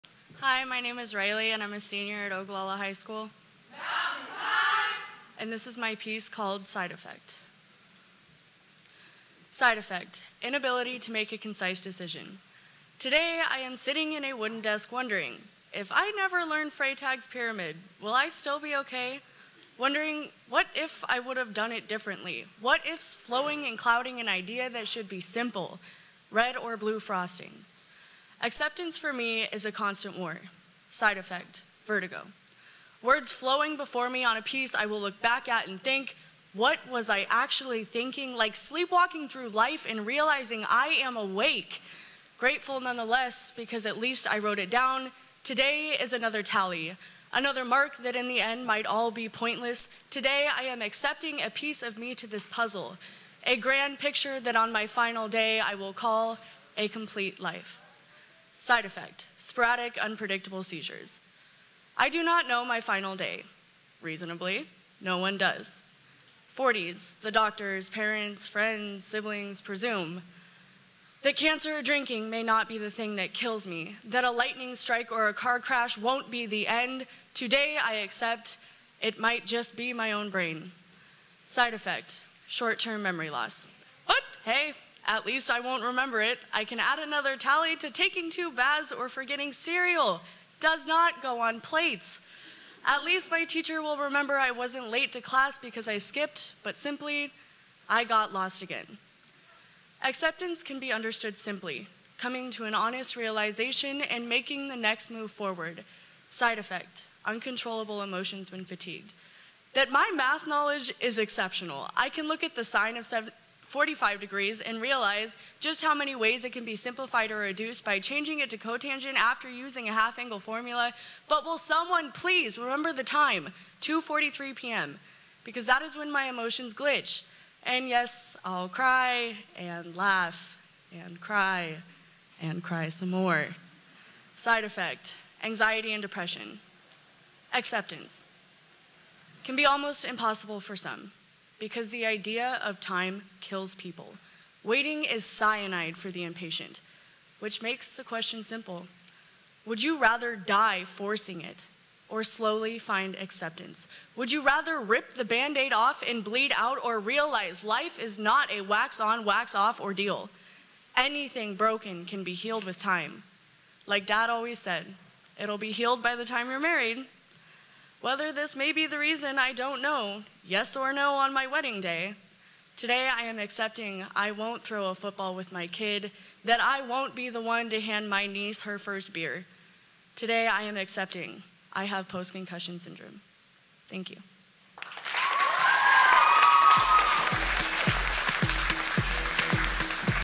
Louder Than a Bomb Great Plains 2019, Bout 9, The Bay, Ogallala
Louder Than a Bomb Great Plains 2019 Bout 9 March 28th at The Bay in Lincoln

Tonight features teams from Lincoln East, Ogallala, and Lincoln Northeast.